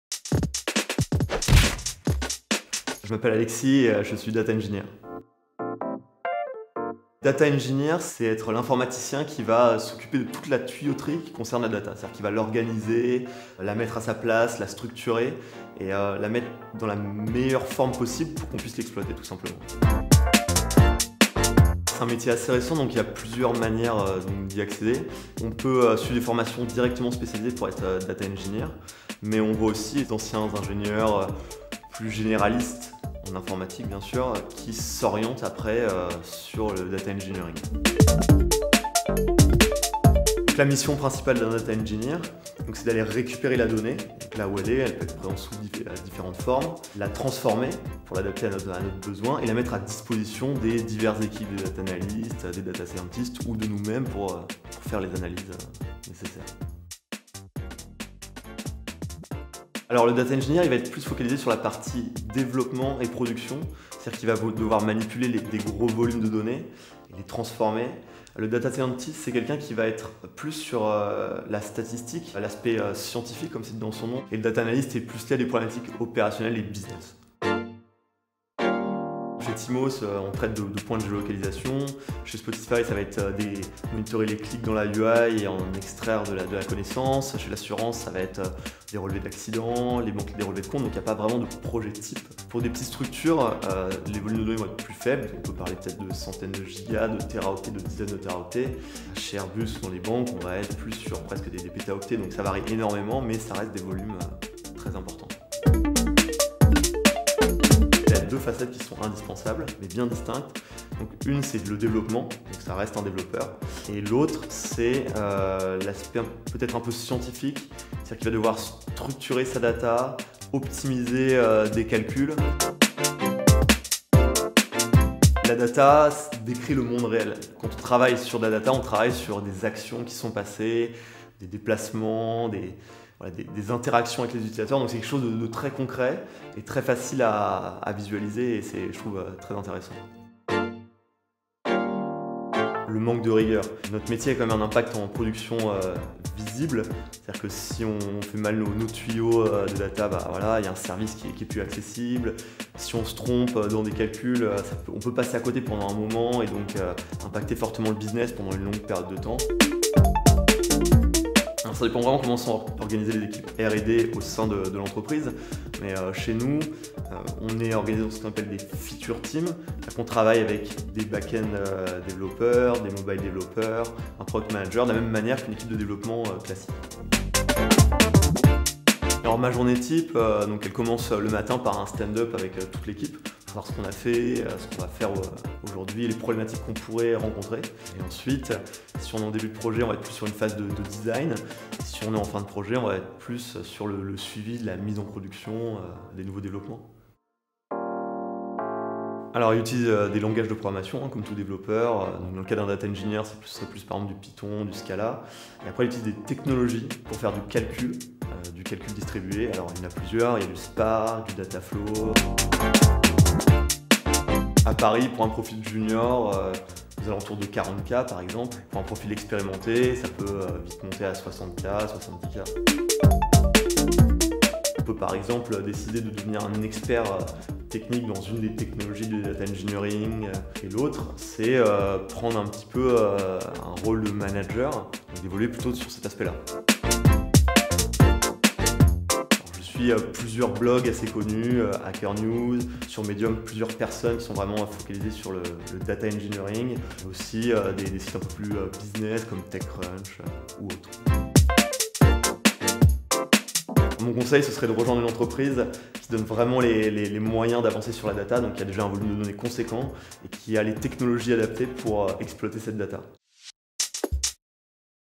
OH MY JOB! : Data Engineer (🇬🇧SUB)
In this episode, Alexis, a data engineer, tells us about his job and day-to-do tasks at Teemo. Data engineers develop, organize, and format large volumes of data so that all the teams in their companies can access them. A little bit architect, a little bit plumber, he creates the structure for using data.